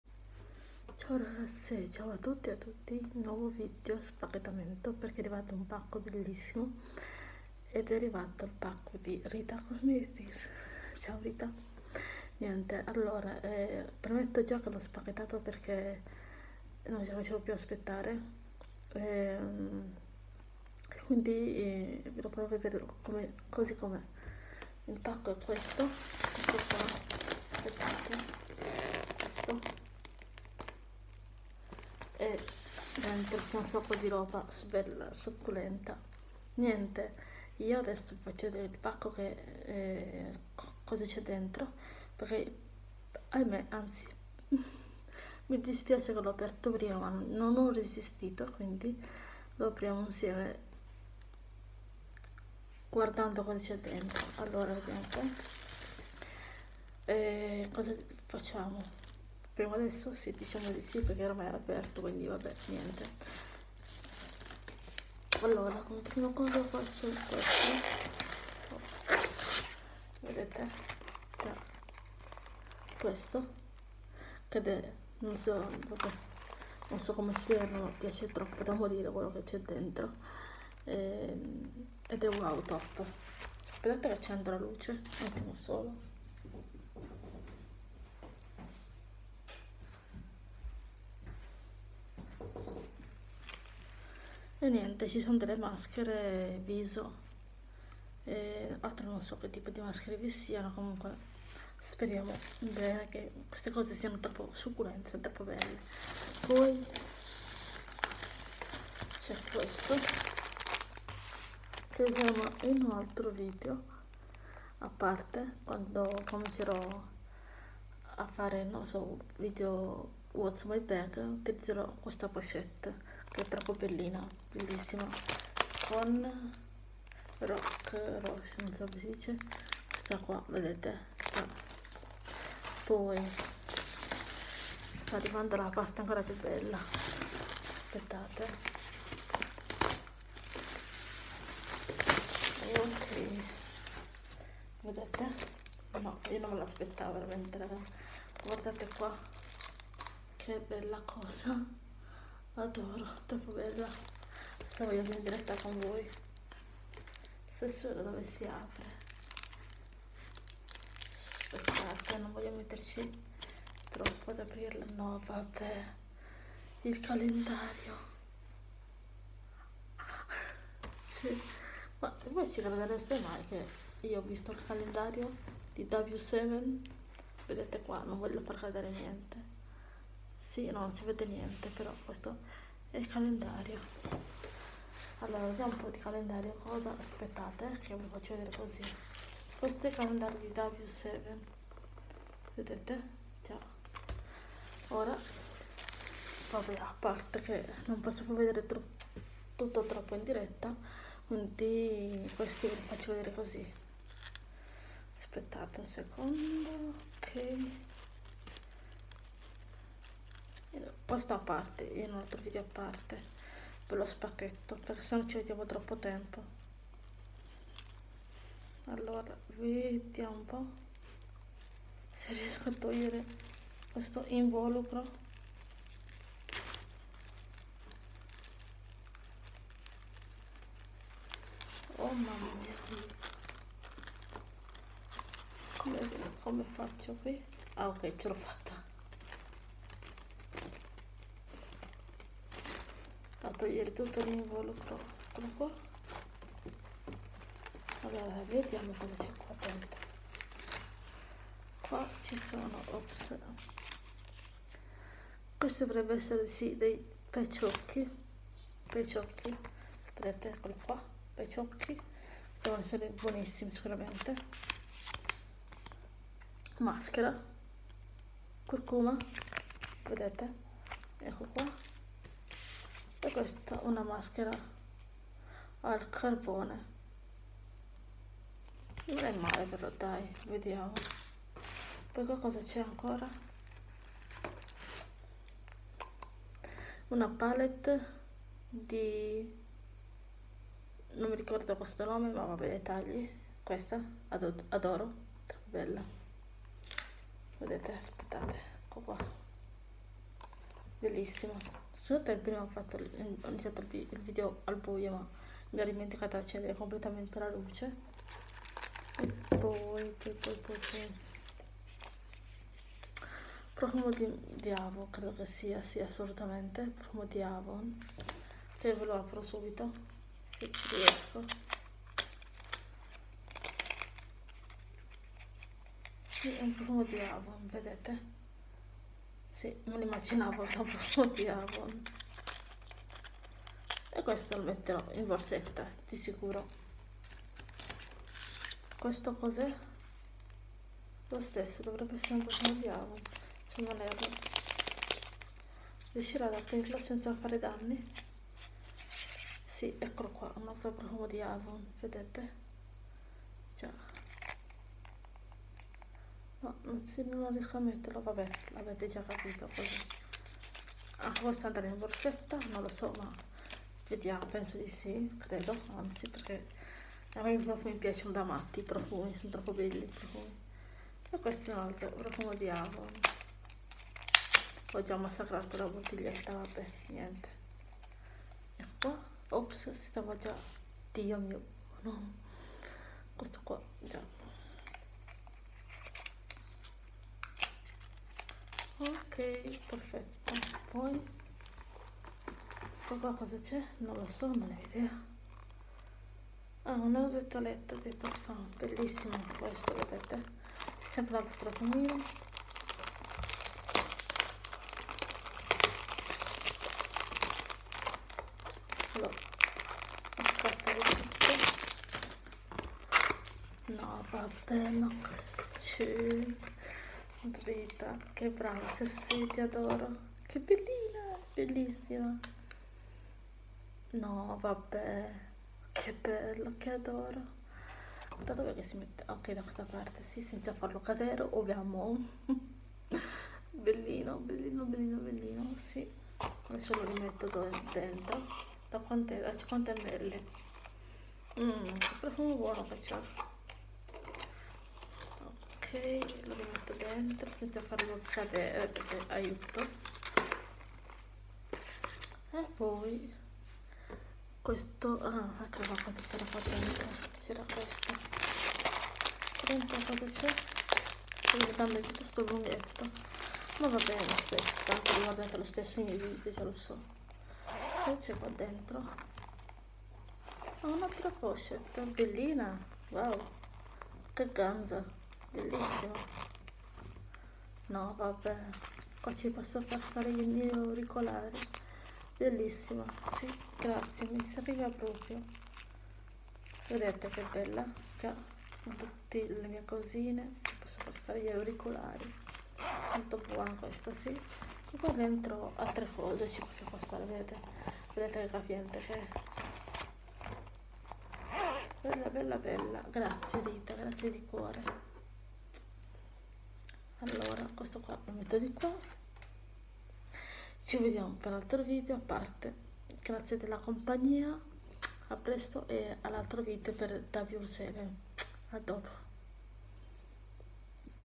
📦🤩unboxing di Rita🎁
Ciao ragazze nuovissimo video spacchettamento
Spero che vi piaccia
Da un amica di YouTube
rita cosmetica
Calendario dell’avvento w7 cosmetics
Maschere skincare viso
Idratazione pach occhi 👀
Al cetriolo 🥒
Pochette 2
Una grande e una piccola
Profumi avon
E profumi di grande dimensione
Palette di ombretti 6